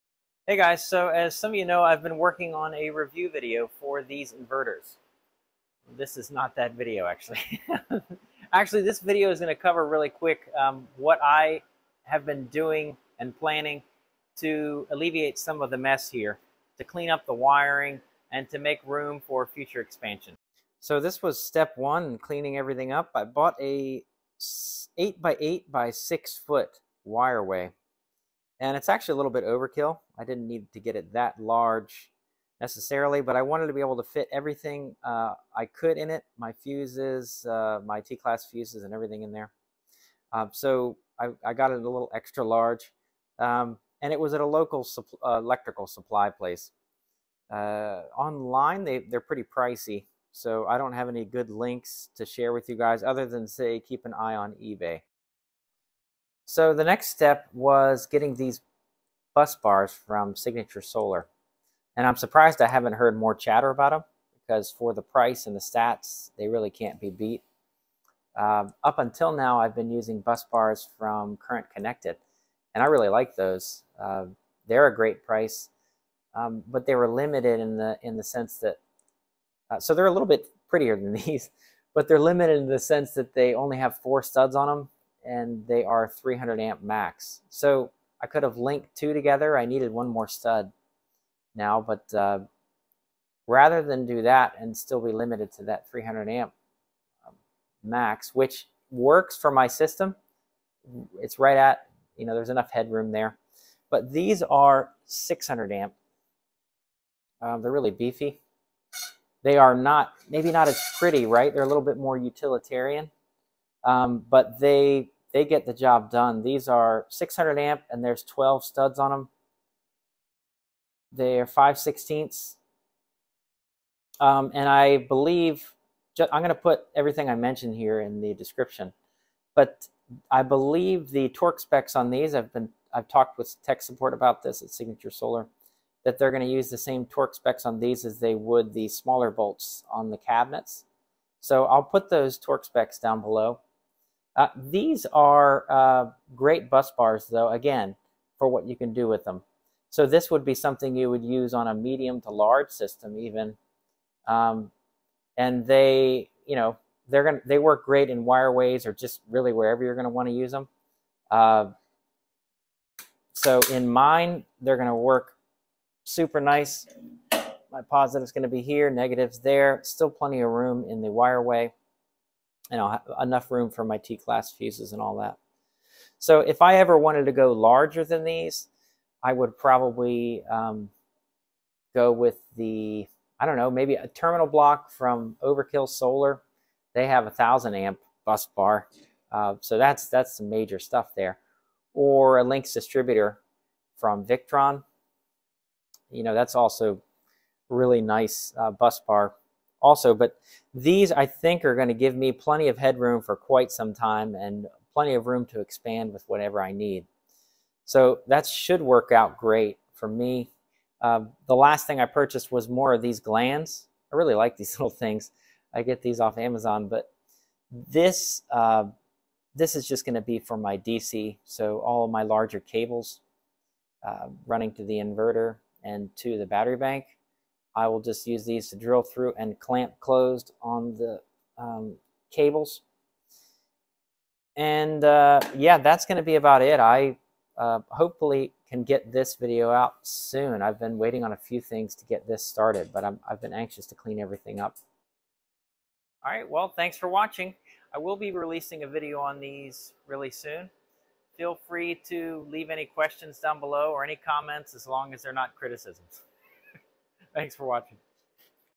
My new favorite bus bar! And preparing to clean things up.
In this video I describe what I’m doing to prepare for a wire way install. And I describe the items I’ve purchased. And of course my new favorite bus bars from signature solar.

600 amp bus bars

Torque specs 8–10 ft lb

Current connected 300 amp bus bars

Overkill solar bus bar/terminal block

Current connected: a quality place to purchase a solark, MPP lv6548 and more.

These are the eg4 rack batteries I use from signature solar.

This is the link to my bms I’m using in one of my packs. Really like it.

Watts247 combiner boxes that I use.

These are the wire glands I mentioned

This is where I got the heavy gauge wire for my system.